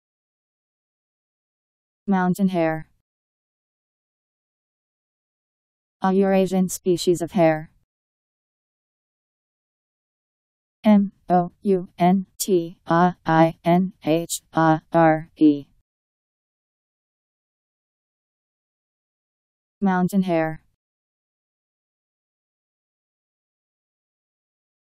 Mountain hare Meaning
Video shows what mountain hare means. A Eurasian species of hare, ..  Mountain hare Meaning. How to pronounce, definition audio dictionary. How to say mountain hare. Powered by MaryTTS, Wiktionary